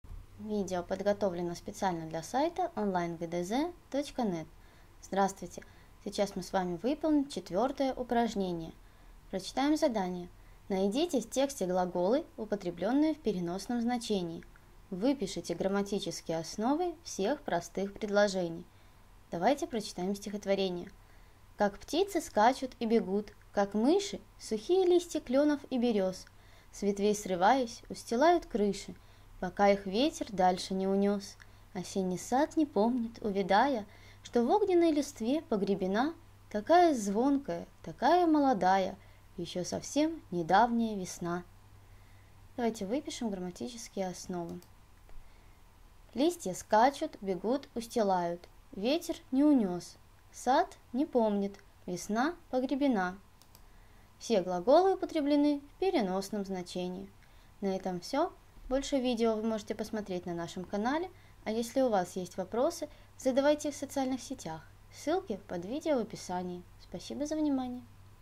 Задание № 4 — Русский язык 7 класс (Ладыженская, Баранов, Тростенцова)
Для выполнения 4 упражнения необходимо внимательно прочитать и найти в тексте глаголы, употребленные в переносном значении. Далее выписать грамматические основы всех простых предложений. Справиться с этим упражнением будет легко, если сначала вы посмотрите видео с подробным объяснением.